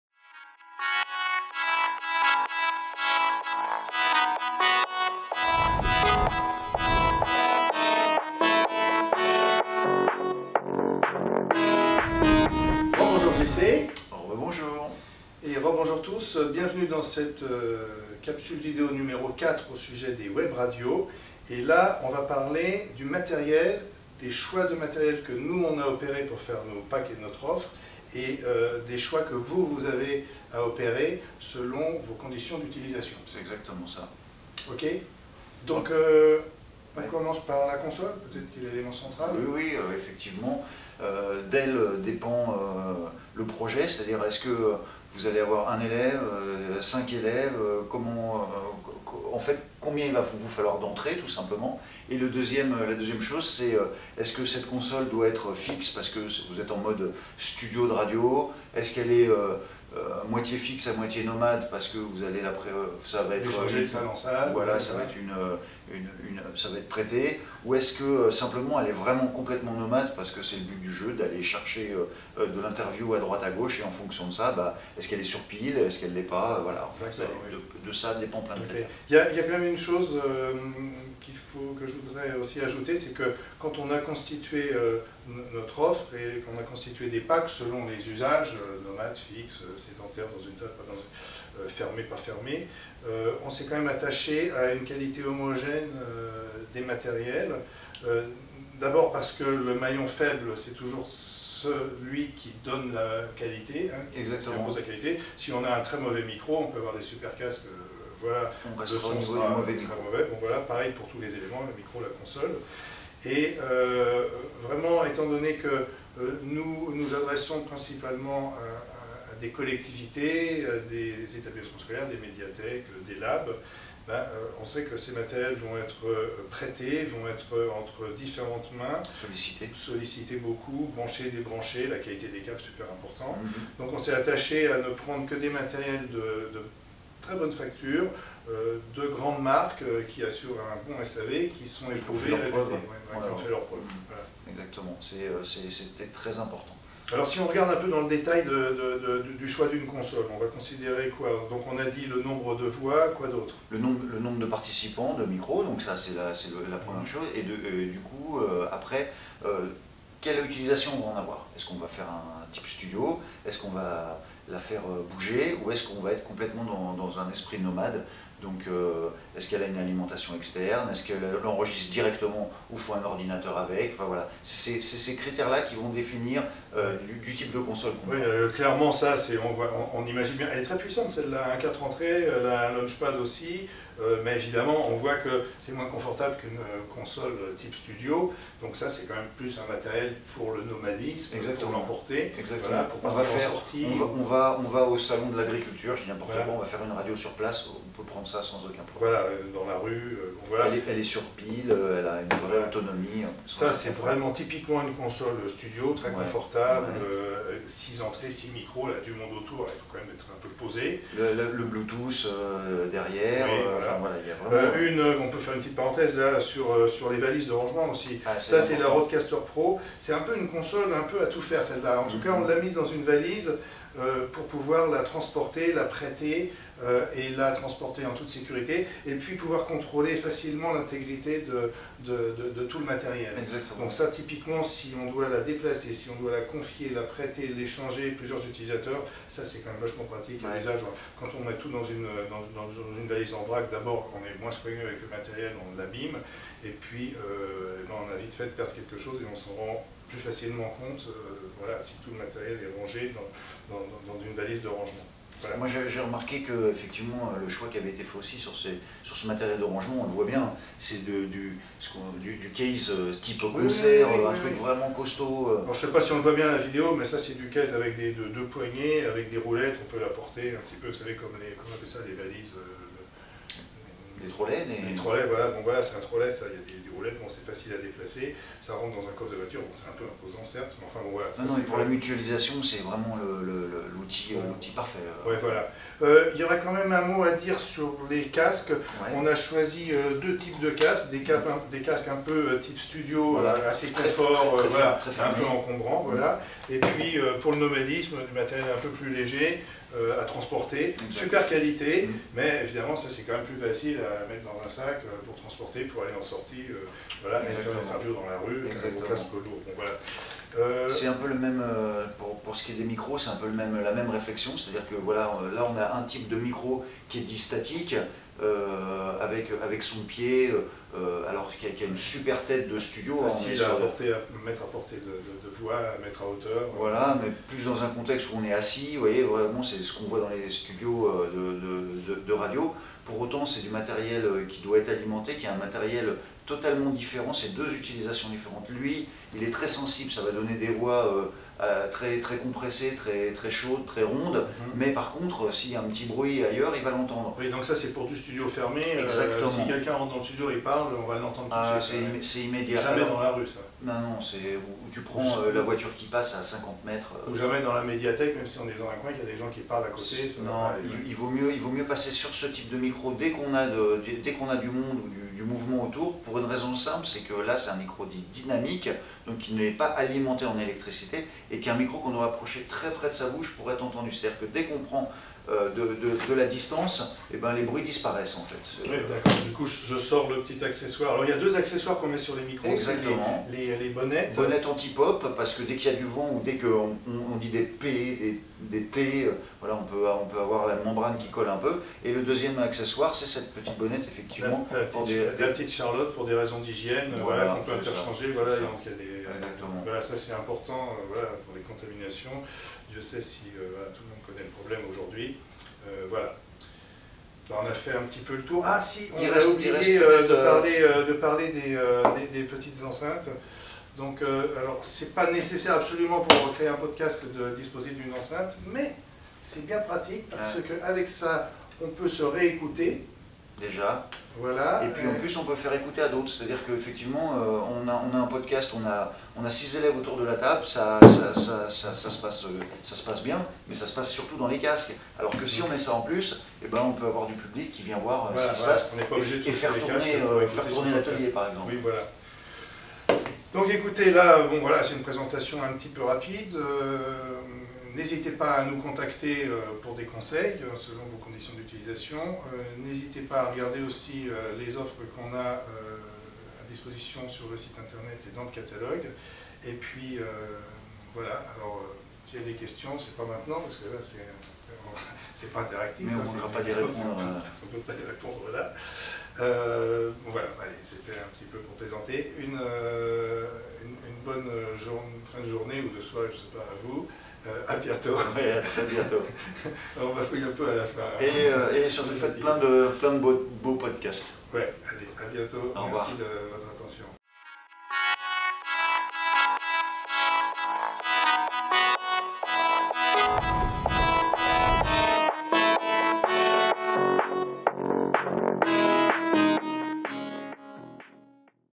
Webradio 4/5 | Comment choisir son matériel de Webradio ?
Parce que les besoins ne sont pas les mêmes selon qu'une Webradio est réalisée en solo ou en groupe, en studio ou sur le terrain, en présentiel ou en distanciel, avec du matériel partagé ou non... il convient de cibler le bon matériel pour le bon contexte.
Nos experts, Étienne et JC, vous invitent à vous poser les bonnes questions !

LES CHAPITRES
00:00:35 La console de mixage
00:03:34 Les solutions de rangement et de protection
00:04:45 Casques et microphones